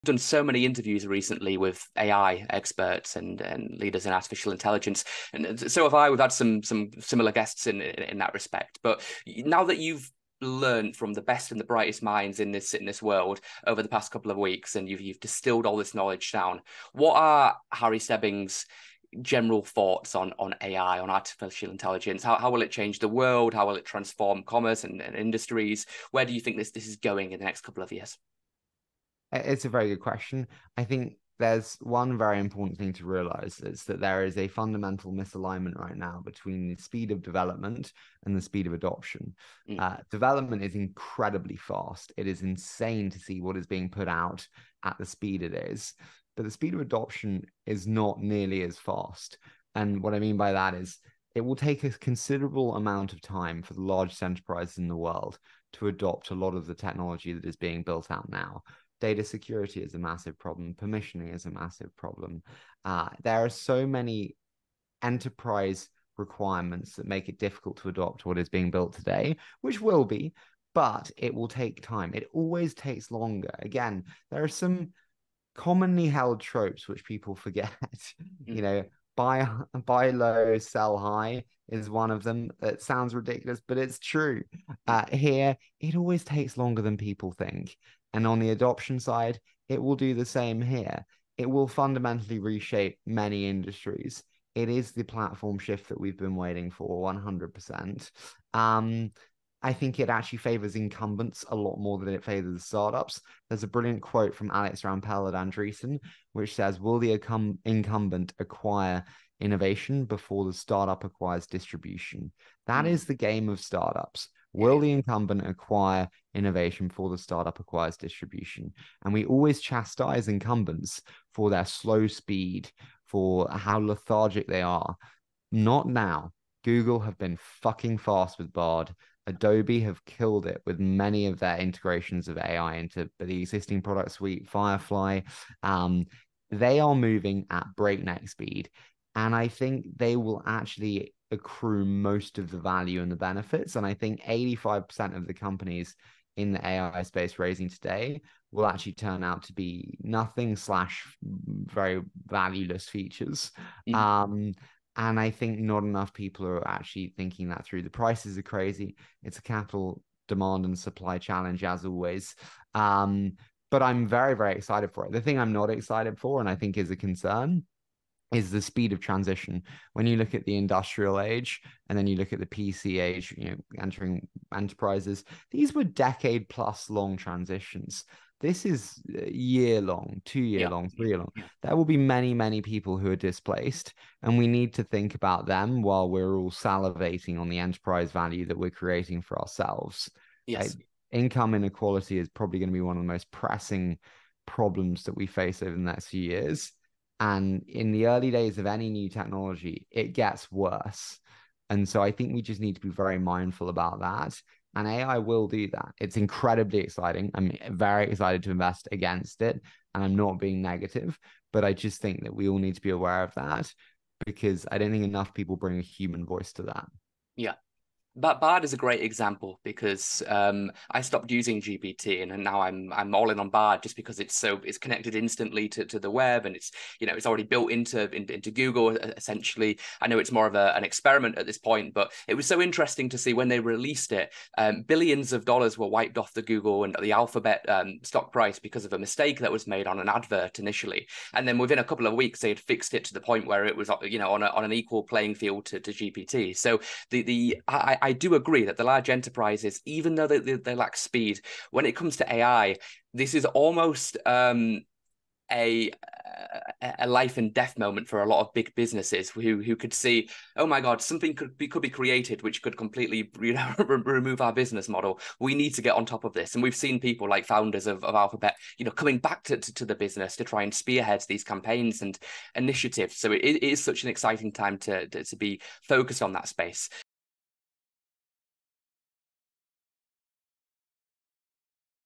Harry Stebbings: The Future of AI
Harry Stebbings is a venture capitalist and podcast host.